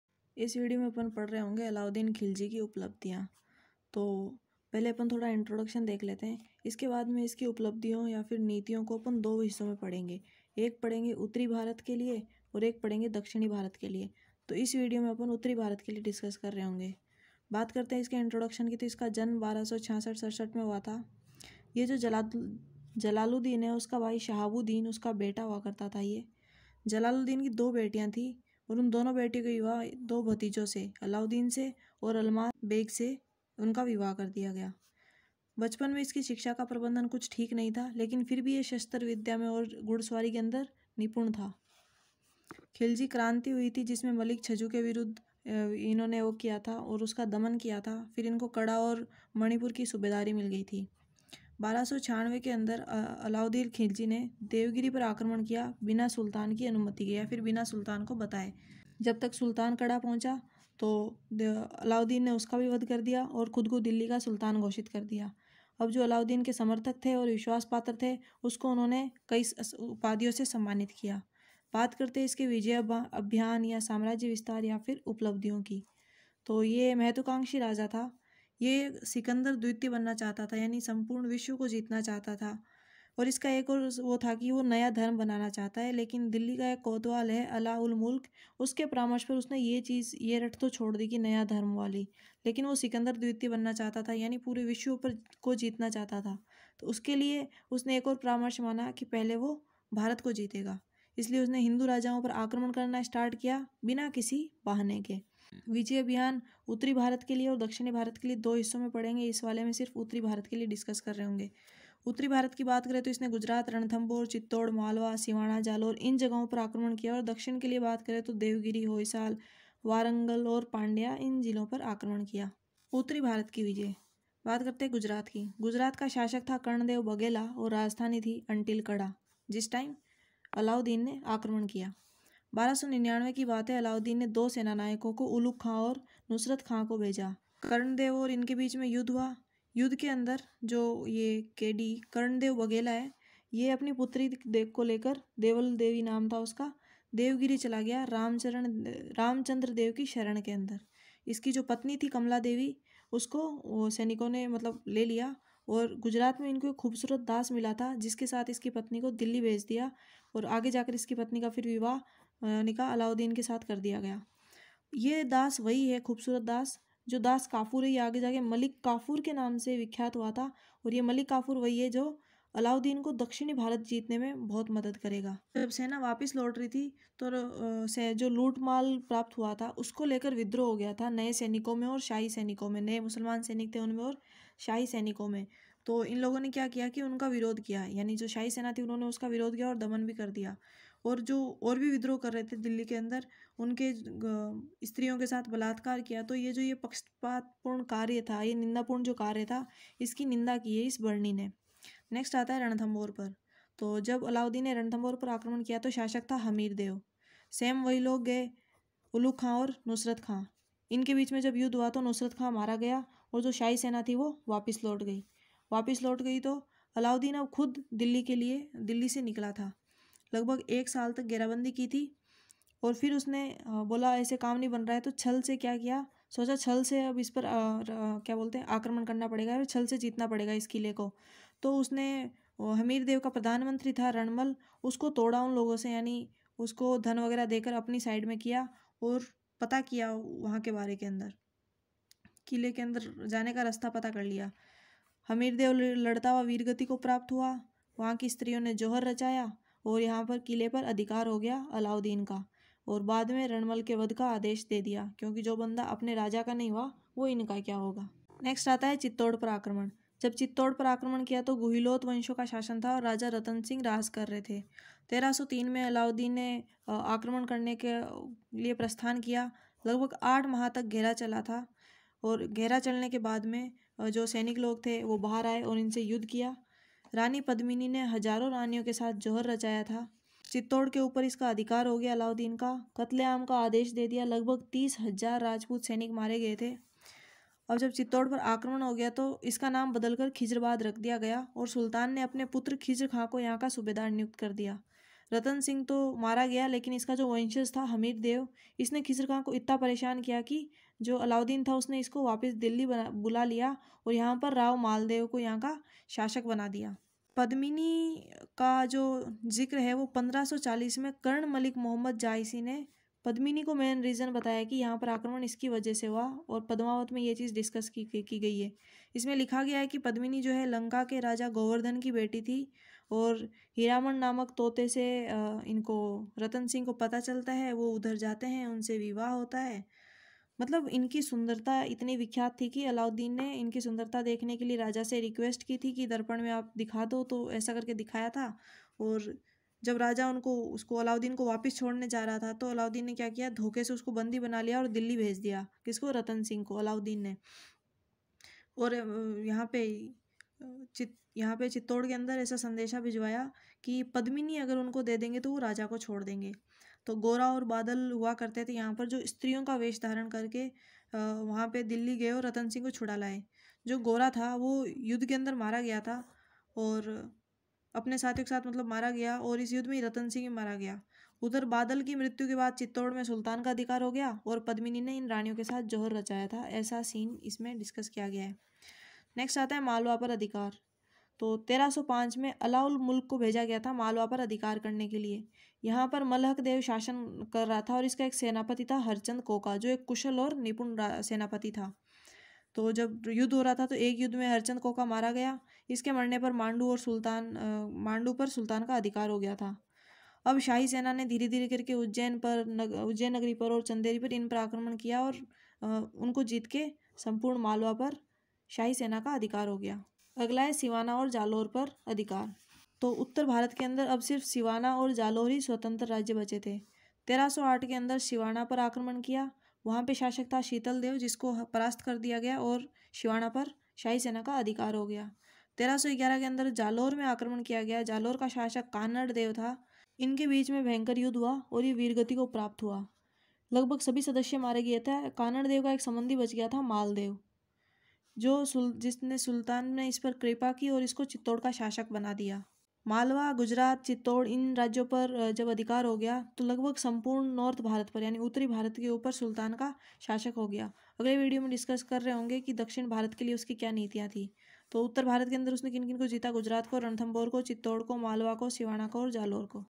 अलाउद्दीन खिलजी की उपलब्धियां।। खिलजी द्वारा किये गए राजस्व सुधार
Achievements of Alauddin Khilji in hindi
अलाउद्दीन खिलजी की उपलब्धियां